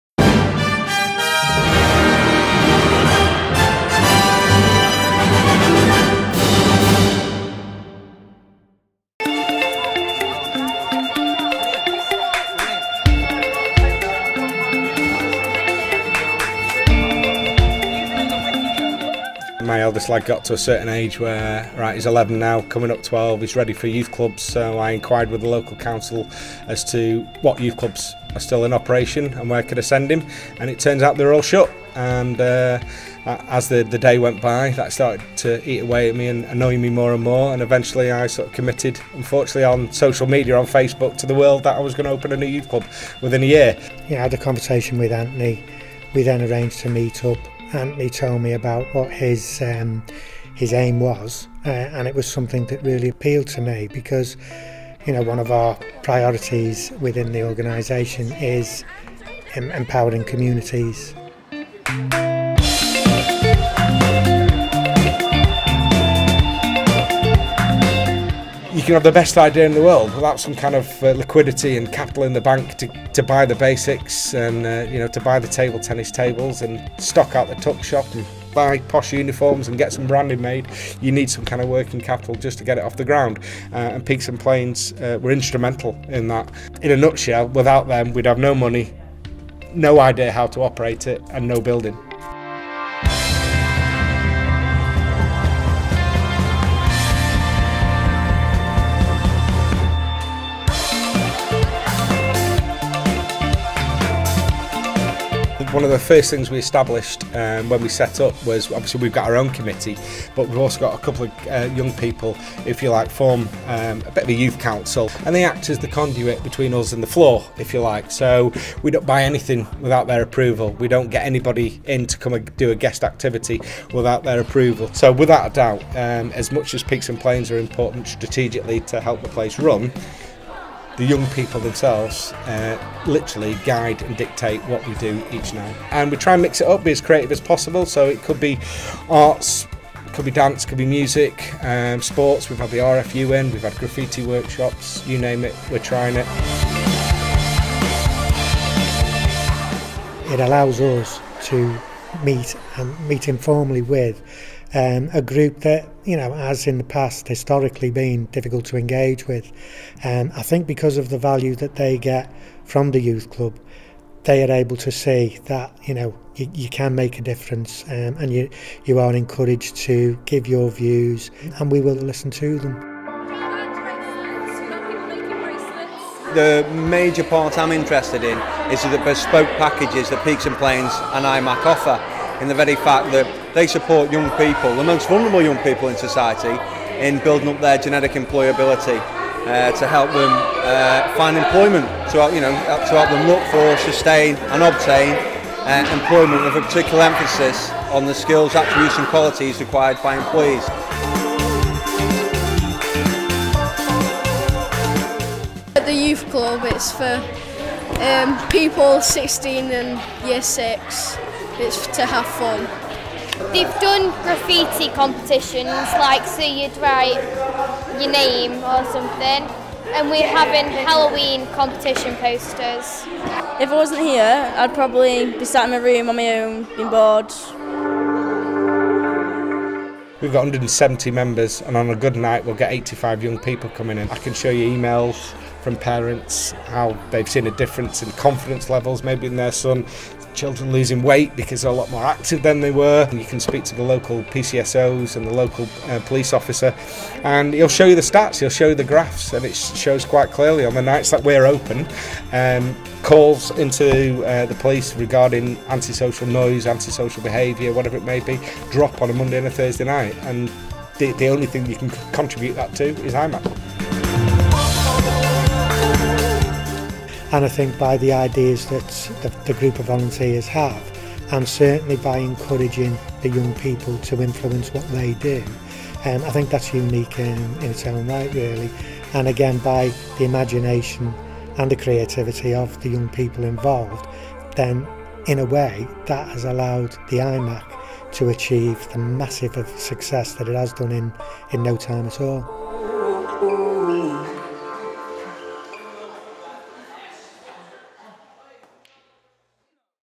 Northern Silver Screen 2013 Peaks and Plains
Northern Silver Screen 2013 finalist in the Creativity category with a special award presented to Antony Allen from iMacc Youth Club.